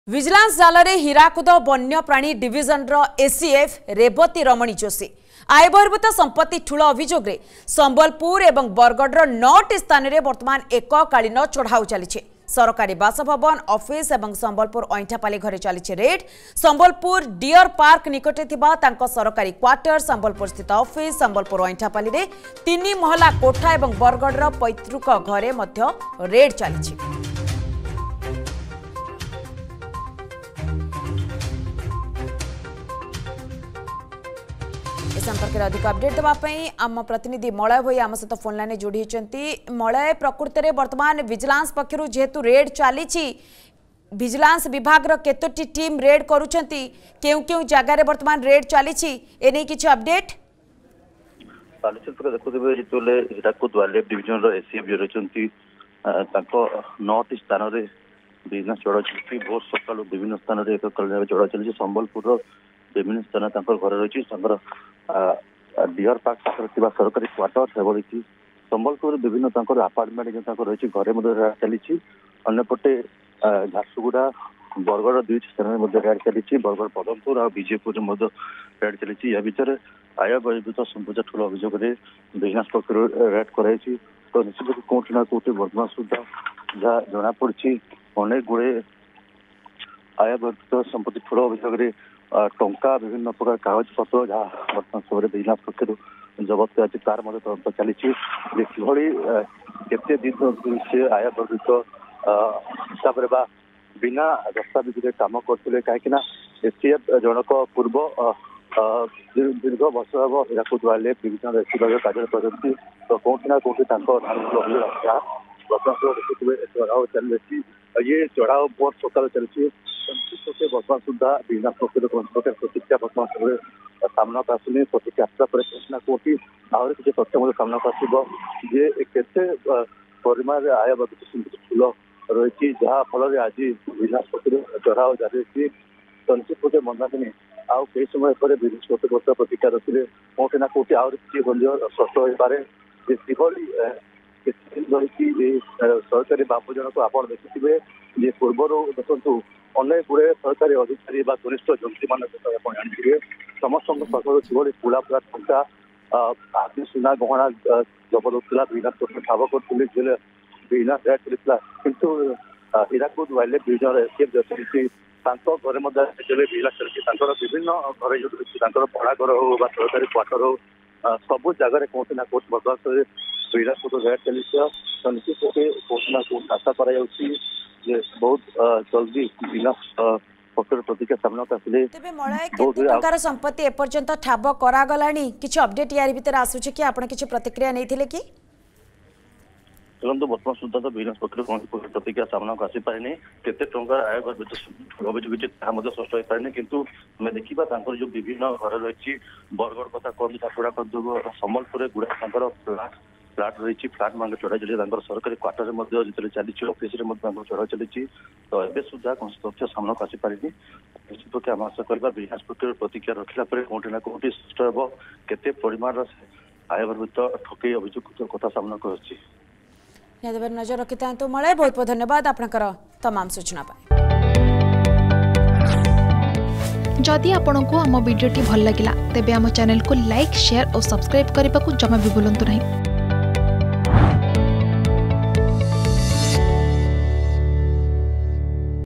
ଭିଜିଲାନ୍ସ ଜାଲରେ ଏସିଏଫ୍ ରେବତୀ ରମଣ ଯୋଶୀ | Vigilance Raid | ACF Revati Raman Joshi
ଭିଜିଲାନ୍ସ ଜାଲରେ ହୀରାକୁଦ ବନ୍ୟପ୍ରାଣୀ ଡିଭିଜନର ଏସିଏଫ୍ ରେବତୀ ରମଣ ଯୋଶୀ ।